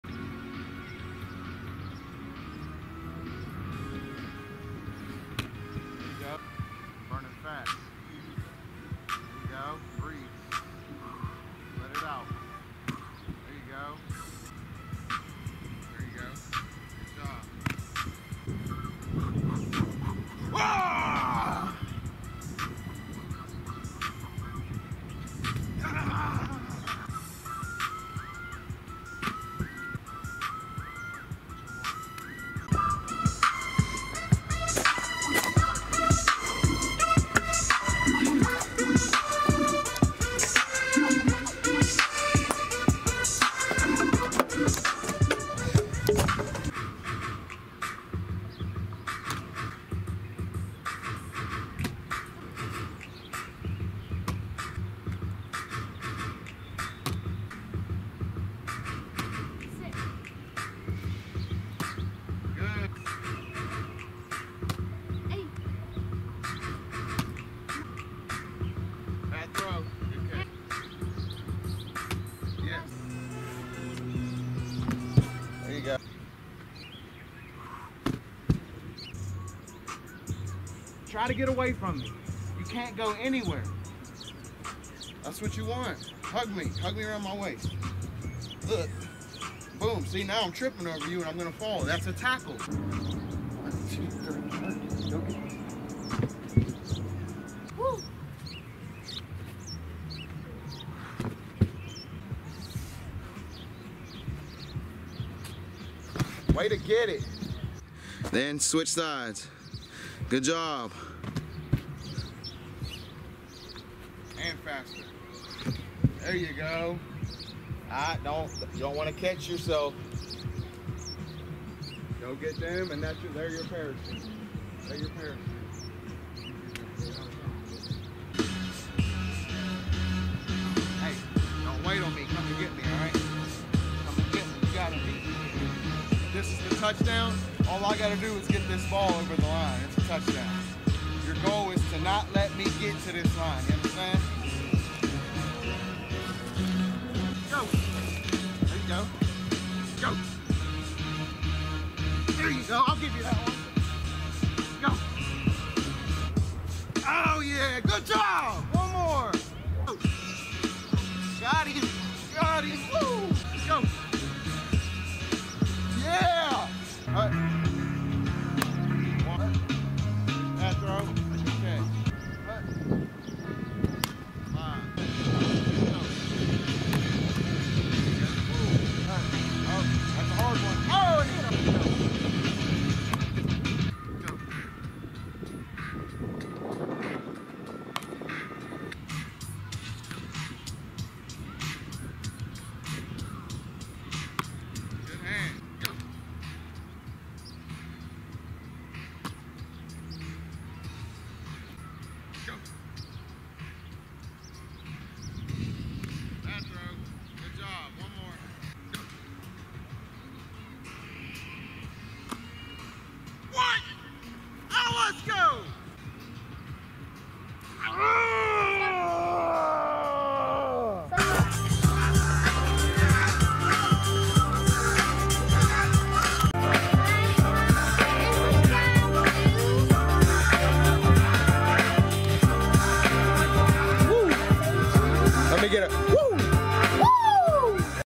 NORTH CHARLESTON WORKOUT 22 JUNE 2024
He started out really stiff, and loosened up as we went along. It was HOT-hot today, which did not help.
Got a lot of work in